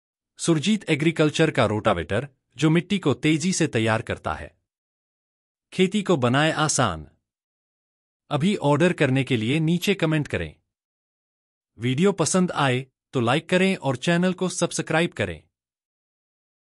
सुरजीत एग्रीकल्चर रोटावेटर: मिट्टी की तैयारी अब तेज और आसान! | SURJEET AGRICULTURE| RAIPUR|ROTAVATOR
सुरजीत एग्रीकल्चर का उन्नत रोटावेटर आपकी मिट्टी को तेजी और समान रूप से तैयार करने में मदद करता है, जिससे आपकी खेती हो जाती है और भी आसान और उत्पादक। अगर आप भी खेती के काम को सरल बनाना चाहते हैं, तो अभी ऑर्डर करें!